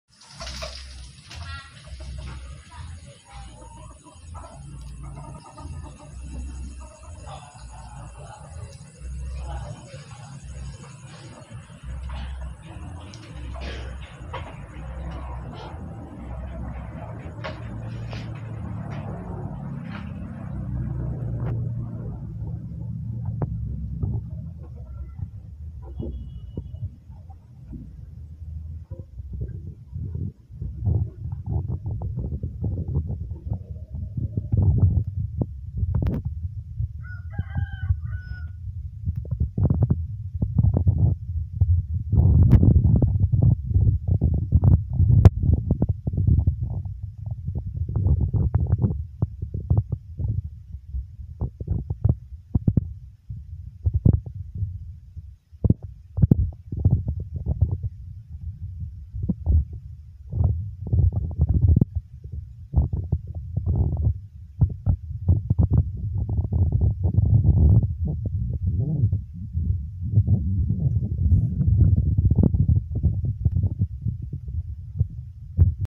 Perjalanan Menuju Pintu Syurga 🕌🕋🕌
Nampak jelas,peroses terbuka 7 pintu syurga dilihat dari   bumi pertiwi.Pengetahuan serta penbah wawasan yang mulia dilangit sang Pencipta Alam Semesta.